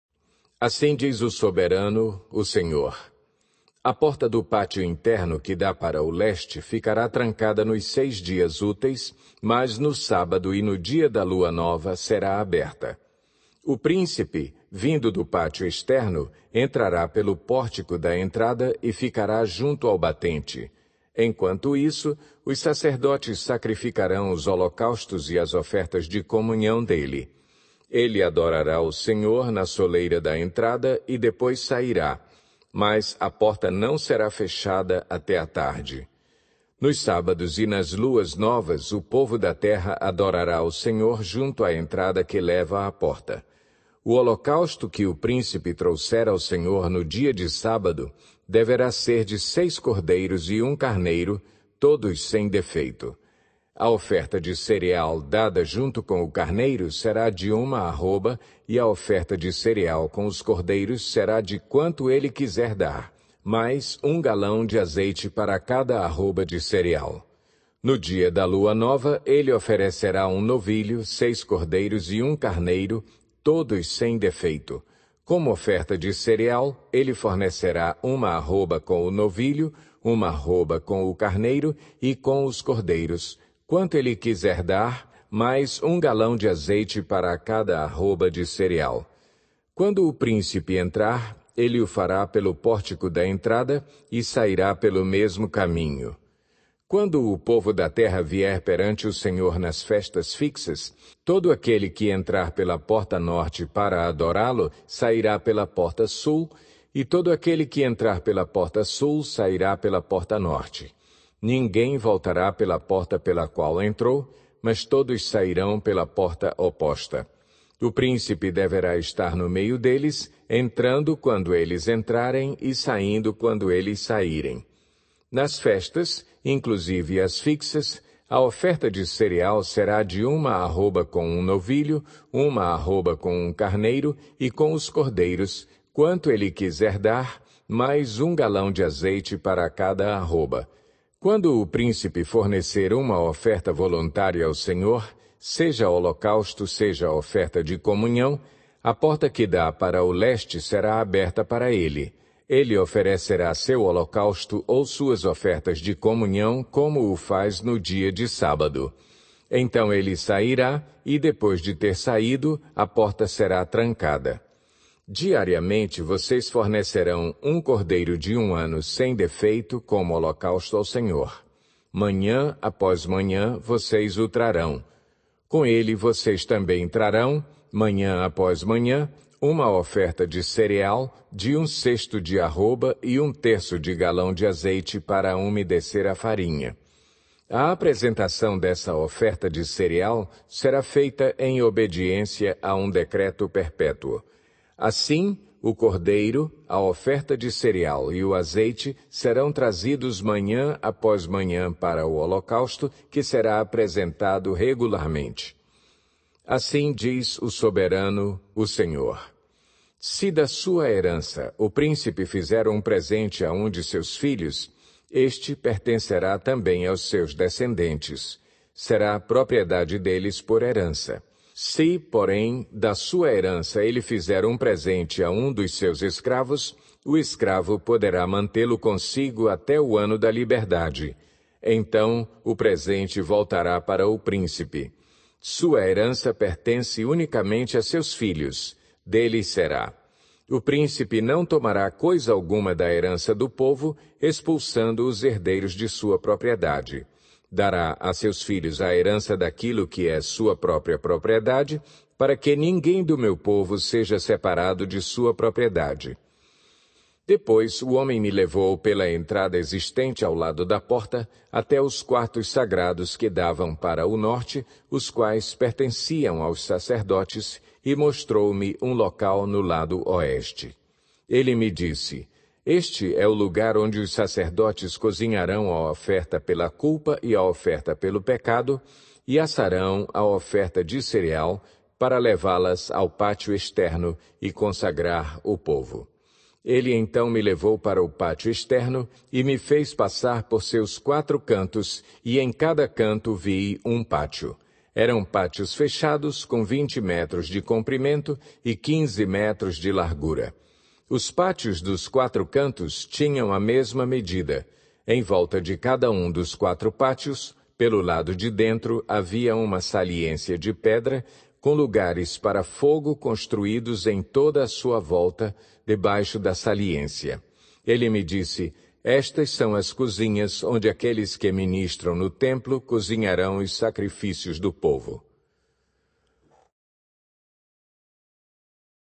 Ezequiel 46
1 - " ‘Assim diz o Soberano Senhor: A porta do pátio interno que dá para o leste ficará trancada nos seis dias úteis, mas no sábado e no dia da lua nova será aberta.
2 - O príncipe, vindo do pátio externo, entrará pelo pórtico da entrada e ficará junto ao batente. Os sacerdotes sacrificarão os holocaustos e as ofertas de comunhão dele. Ele adorará o Senhor na soleira da entrada e depois sairá, mas a porta não será fechada até à tarde.
3 - Nos sábados e nas luas novas o povo da terra adorará o Senhor junto à entrada que leva à porta.
4 - O holocausto que o príncipe trouxer ao Senhor no dia de sábado deverá ser de seis cordeiros e um carneiro, todos sem defeito.
5 - A oferta de cereal dada junto com o carneiro será de uma arroba, e a oferta de cereal com os cordeiros será de quanto ele quiser dar, mais um galão de azeite para cada arroba de ceral.
6 - No dia da lua nova ele oferecerá um novilho, seis cordeiros e um carneiro, todos sem defeito.
7 - Como oferta de cereal ele fornecerá uma arroba com o novilho, uma arroba com o carneiro, e com os cordeiros, quantos ele quiser dar, e mais um galão de azeite para cada uma arroba de cereal.
8 - Quando o príncipe entrar, ele o fará pelo pórtico da entrada, e sairá pelo mesmo caminho.
9 - " ‘Quando o povo da terra vier perante o Senhor nas festas fixas, todo aquele que entrar pela porta norte para adorá-lo sairá pela porta sul, e todo aquele que entrar pela porta sul sairá pela porta norte. Ninguém voltará pela porta pela qual entrou, mas todos sairão pela porta oposta.
10 - O príncipe deverá estar no meio deles, entrando quando eles entrarem e saindo quando eles saírem.
11 - " ‘Nas festas, inclusive nas festas fixas, a oferta de cereal será de uma arroba com um novilho, uma arroba com um carneiro, e de uma arroba com os cordeiros, quantos ele quiser dar, mais um galão de azeite para cada arroba.
12 - Quando o príncipe fornecer uma oferta voluntária ao Senhor, seja holocausto seja oferta de comunhão, a porta que dá para o leste será aberta para ele. Ele oferecerá seu holocausto ou suas ofertas de comunhão como o faz no dia de sábado. Então ele sairá, e, depois de ter saído, a porta será trancada.
13 - " ‘Diariamente vocês fornecerão um cordeiro de um ano sem defeito como holocausto ao Senhor; manhã após manhã vocês o trarão.
14 - Com ele vocês também trarão, manhã após manhã, uma oferta de cereal, de um sexto de arroba e um terço de galão de azeite para umedecer a farinha. A apresentação dessa oferta de cereal será feita em obediência a um decreto perpétuo.
15 - Assim o cordeiro, a oferta de cereal e o azeite serão trazidos manhã após manhã para o holocausto que será apresentado regularmente.
16 - " ‘Assim diz o Soberano Senhor: Se da sua herança o príncipe fizer um presente a um de seus filhos, ele pertencerá também aos seus descendentes; será propriedade deles por herança.
17 - Se, porém, da sua herança ele fizer um presente a um dos seus escravos, o escravo poderá mantê-lo consigo até o ano da liberdade; então ele voltará para o príncipe. Sua herança pertence unicamente a seus filhos; deles será.
18 - O príncipe não tomará coisa alguma da herança do povo, expulsando-os de sua propriedade. Dará a seus filhos a herança daquilo que é sua própria propriedade, para que ninguém do meu povo seja separado de sua propriedade’ ".
19 - Depois o homem me levou, pela entrada ao lado da porta, até os quartos sagrados que davam para o norte, os quais pertenciam aos sacerdotes, e mostrou-me um local no lado oeste.
20 - Ele me disse: "Este é o lugar onde os sacerdotes cozinharão a oferta pela culpa e a oferta pelo pecado e assarão a oferta de cereal, para levá-las ao pátio externo e consagrar o povo".
21 - Ele então me trouxe para o pátio externo e me fez passar por seus quatro cantos, e em cada canto vi um pátio.
22 - Eram pátios fechados, com vinte metros de comprimento e quinze metros de largura; os pátios dos quatro cantos tinham a mesma medida.
23 - Em volta de cada um dos quatro pátios, pelo lado de dentro, havia uma saliência de pedras, com lugares para fogo construídos em toda a sua volta debaixo da saliência.
24 - Ele me disse: "Estas são as cozinhas onde aqueles que ministram no templo cozinharão os sacrifícios do povo".